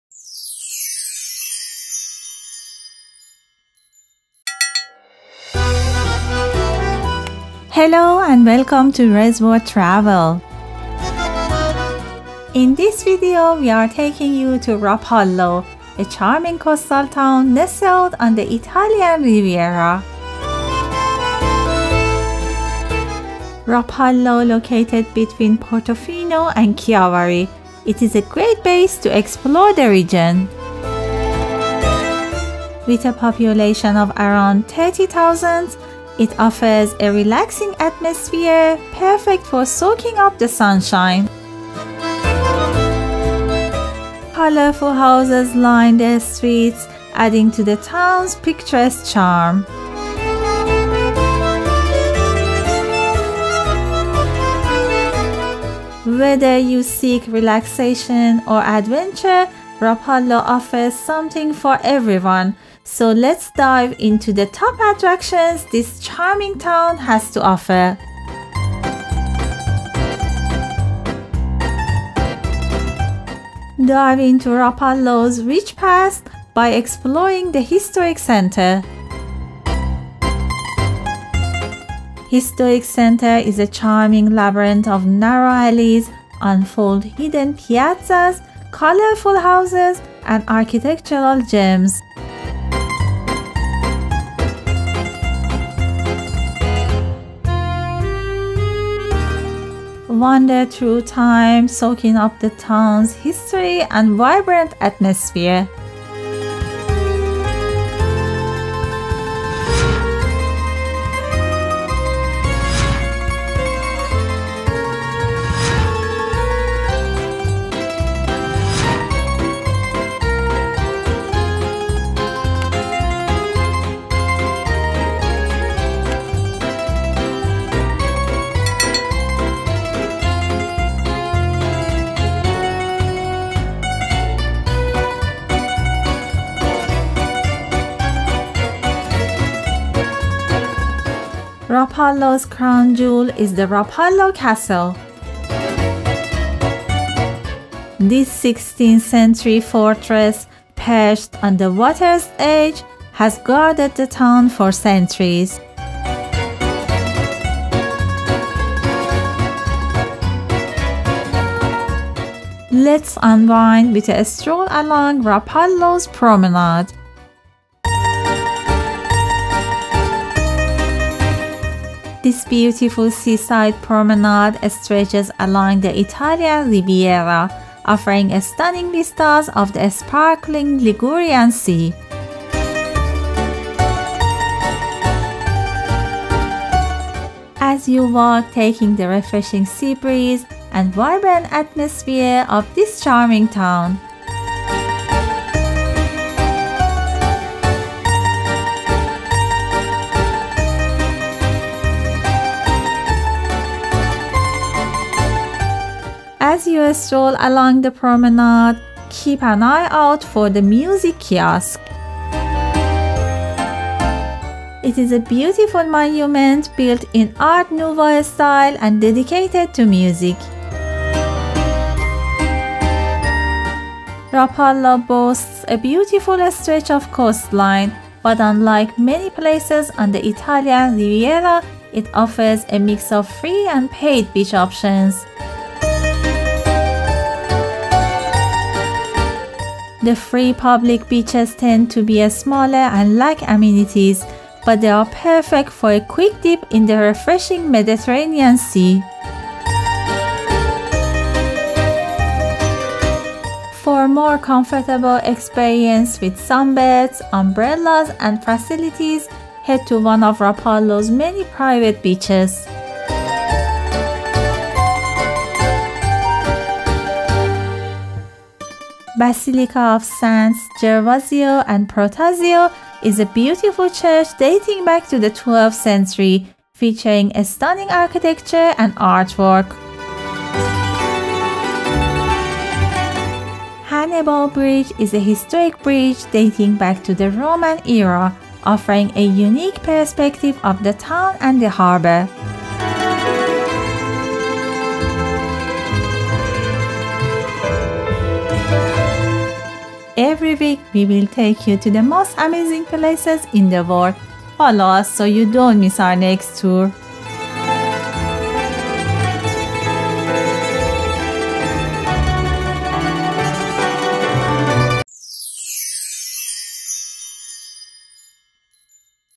[4k] Rapallo Travel Guide 2024 | Rapallo Italian Riviera | Best Cities to visit in Italy
Embark on a captivating journey through the charming Italian town of Rapallo with this visual tour! In this video, we'll unveil the hidden beauty of Rapallo, showcasing:
✨ Breath-taking Sights: Explore Rapallo's stunning harbour, historical landmarks, and picturesque streets.
✨ Local Gems: Discover hidden piazzas, charming alleys waiting to be explored.
✨ Cultural Delights: Immerse yourself in the rich history and culture that Rapallo has to offer.
✨ Have you ever been to Rapallo? Tell us about your experience in the comments below!

👉Share your thoughts and questions in the comments below!

Time Stamp
00:00 Introduction
01:15  Historic Center
02:21 Rapallo Castle
02:44 Rapallo Promenade
03:27  Music Kiosk
03:47 Rapallo beaches
04:31 Basilica of Saints Gervasio and Protasio
04:44 Hannibal Bridge
05:01Monument of Christopher Columbus

👉All material in this video is original and personally recorded by Rez World Travel on location.